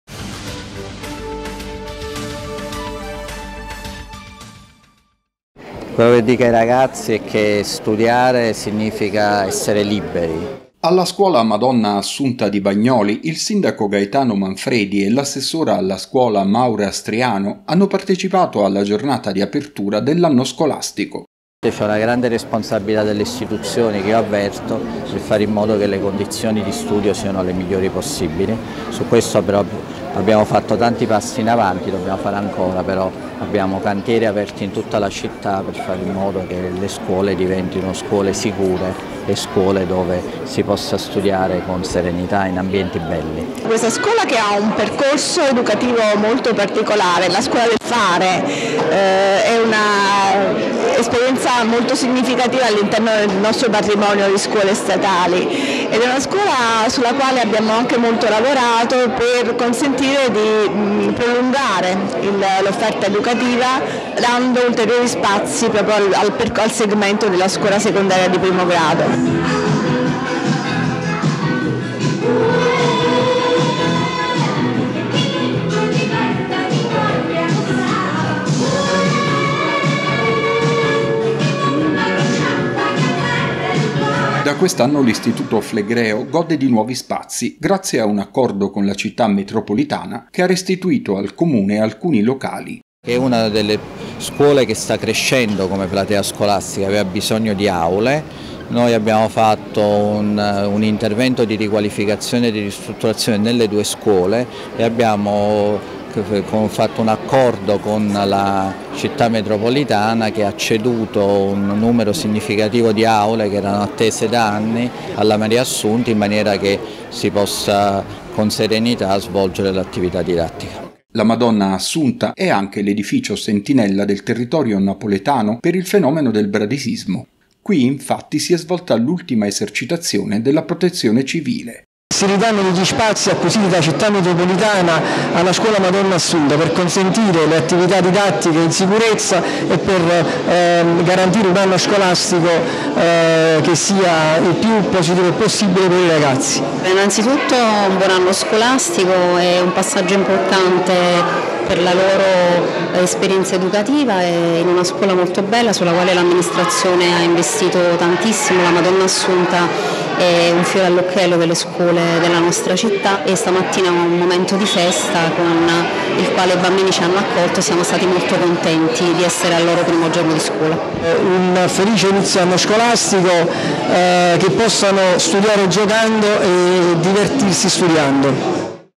Alla Madonna Assunta di Bagnoli il sindaco inaugura l’anno scolastico
Il sindaco Gaetano Manfredi e l’assessora alla scuola Maura Striano hanno partecipato alla giornata di apertura dell’anno scolastico della scuola Madonna Assunta di Bagnoli. Da quest’anno l’istituto flegreo gode di nuovi spazi grazie a un accordo con la Città Metropolitana, che ha ceduto al Comune alcuni locali.